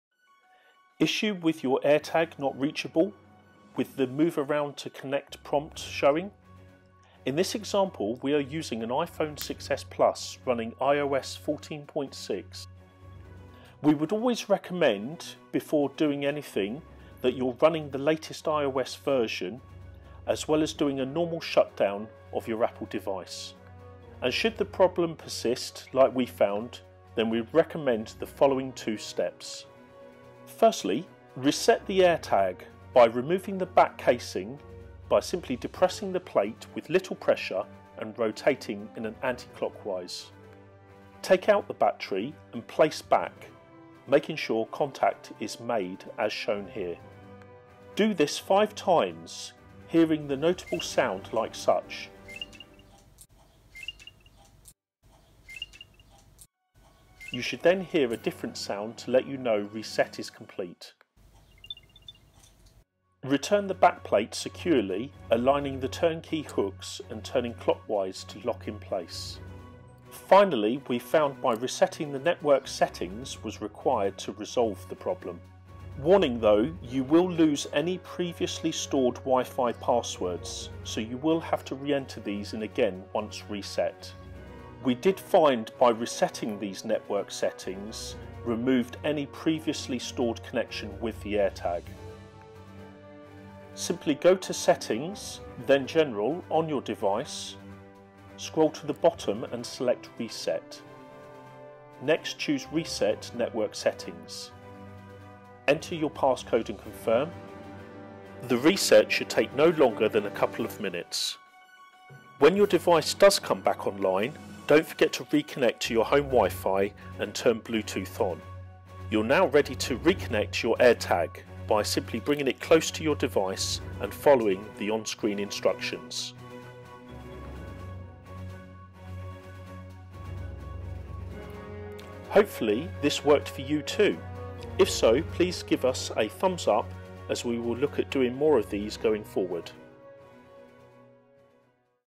AirTag Issue - not reachable - move around to connect - Fix 👍
We wanted to share with you a simple fix we found, when we encountered our new AirTag showing "Not Reachable - move around to connect prompt".

Our two step process we found remedied this issue in under 2 minutes.

0:14 - Basic checks: 1. Update your iOS to 14.6 or above, Bluetooth is turned-on, connected to the internet, and Location Services turned on. And a full shut down and restart-up.
*Should the problem persist like we found, then we would recommend the following TWO steps.
0:31 - Step 1: Reset the AirTag
1:10 - Finally, Reset the Network Settings
1:54 - After re-set, re-connect your AirTag.